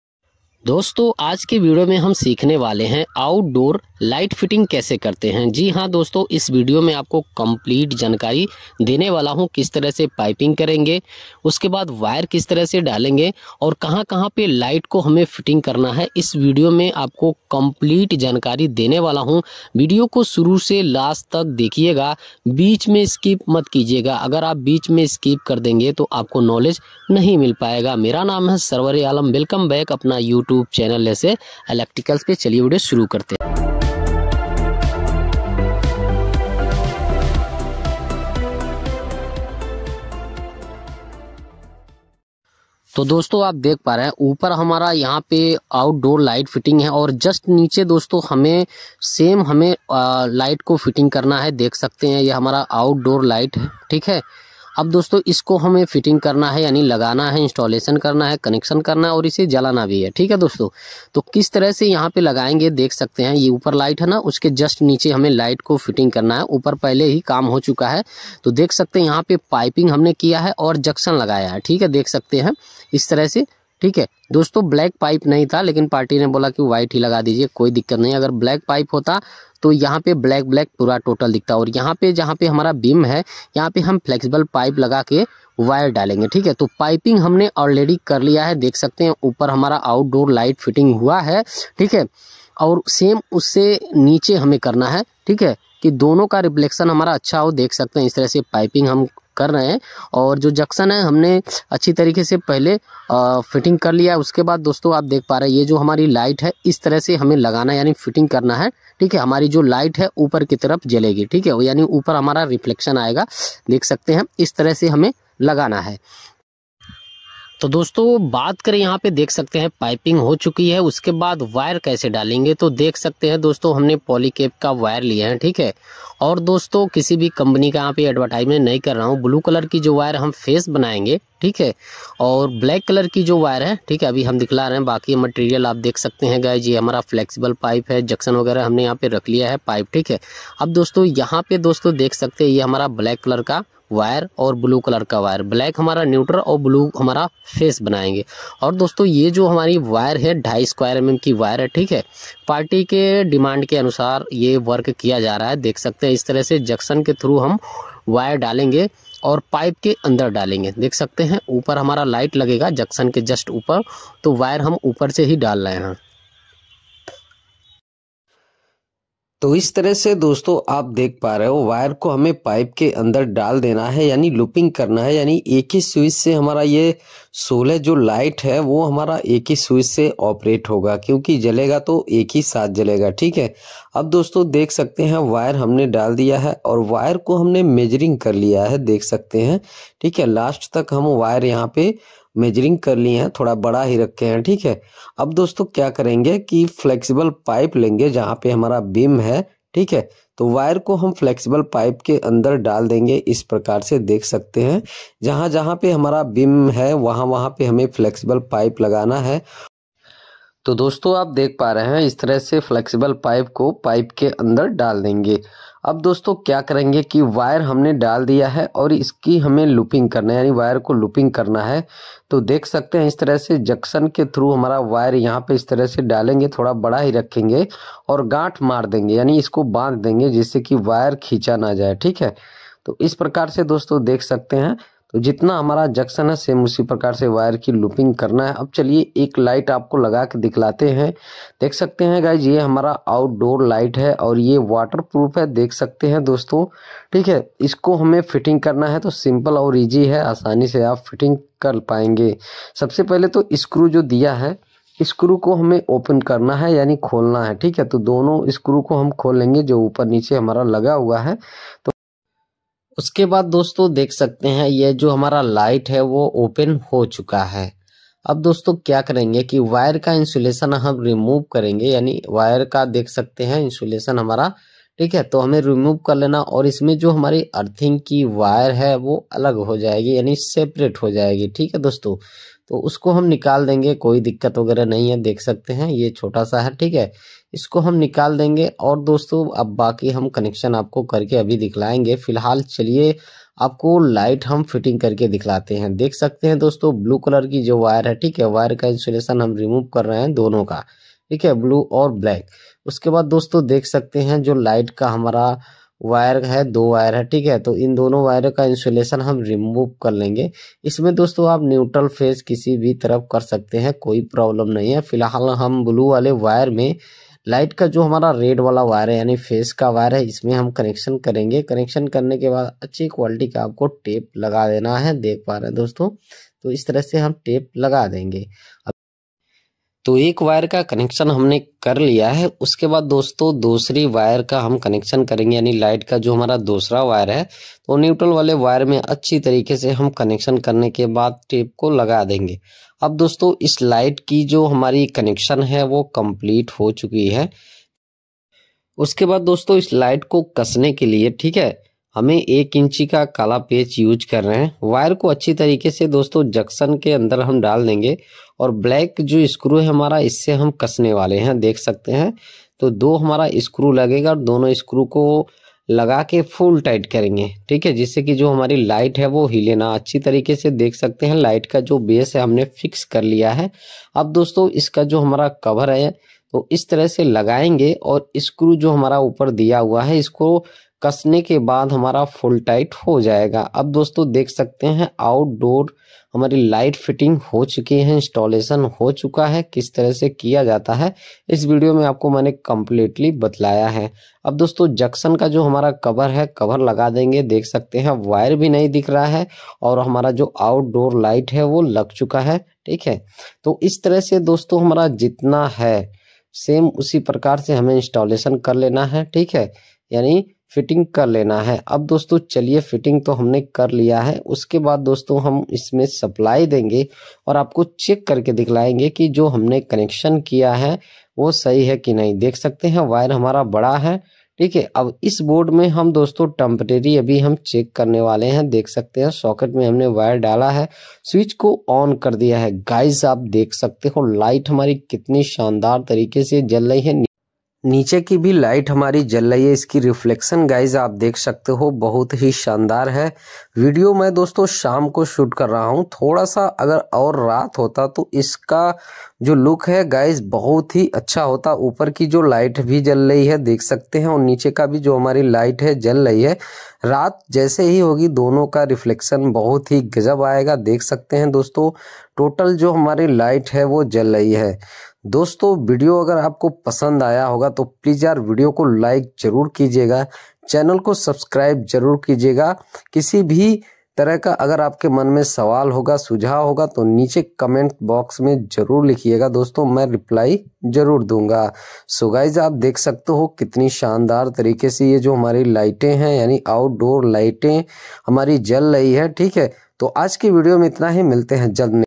Outdoor light fitting installation|how to install outdoor light fitting|outdoor light kaise lagaye
Outdoor light fitting installation||how to outdoor light fitting||outdoor light kaise lagaye||

||About video||
Dosto Aaj k es new video mein outdoor light fitting installation kaise karte hai complete knowledge mil jaayegi||

Guy's please watching more videos some links